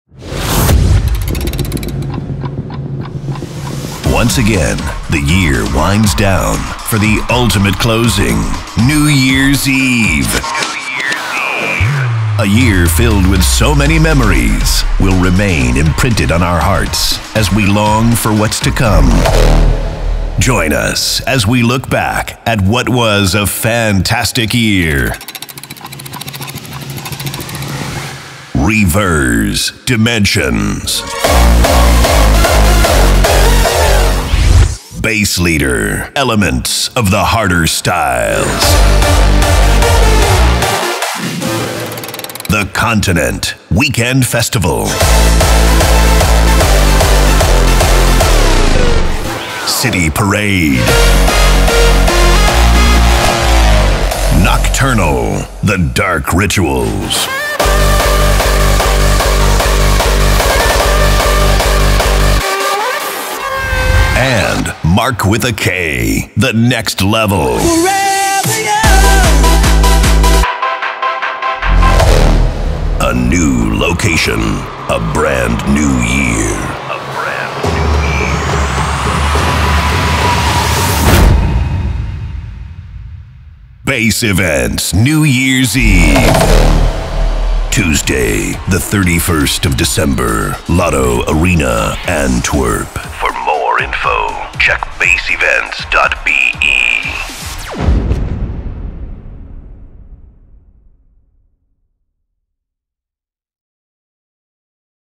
Bass Events NYE (Official 2013 Trailer)
We would like to close of a spectacular 2013 and welcome a hopefully amazing 2014 with you all on the 31st of December at Bass Events NYE!

This year, we'll be taking over the Lotto Arena in Antwerp including a 2nd area for you to choose from!

Coone / Zatox / Frontliner / Lords of TEK / Da Tweekaz / Ran-D / Adaro / Evil Activities / Hard Driver / Ruthless / Dr Rude / Phuture Noize Live / Dr Phunk / Dark-E / Pat B / Lowriderz / Kid Rascal / MC Chucky / MC DL

Regular: 32,50 EUR ex fee
VIP: SOLD OUT (VIP entrance, VIP deck and a glass of Bubbles to celebrate!)

Min Age: 16